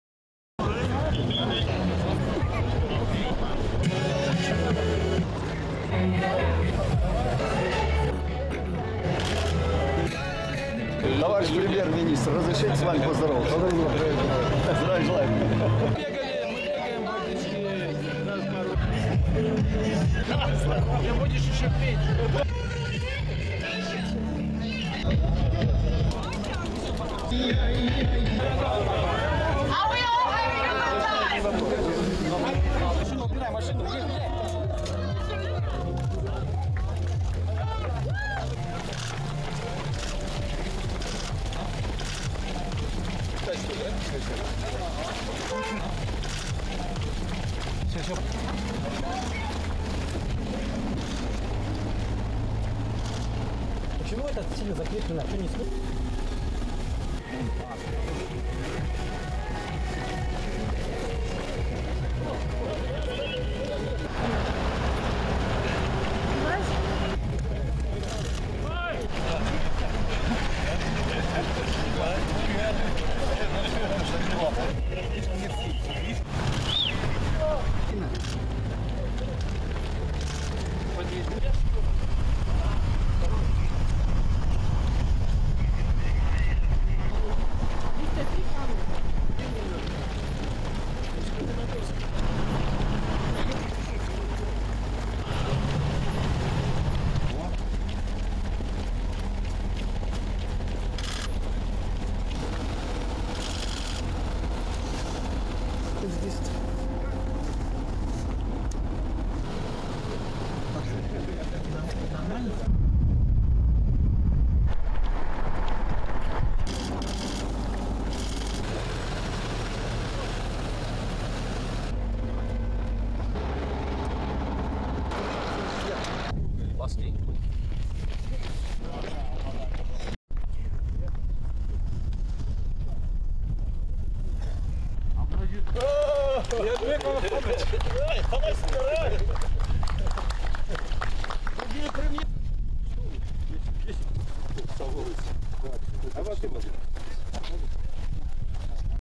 Карим Масимов - благотворительный марафон
Премьер-Министр РК Карим Масимов принял участие в первом благотворительном марафоне BKS Эйр Астана.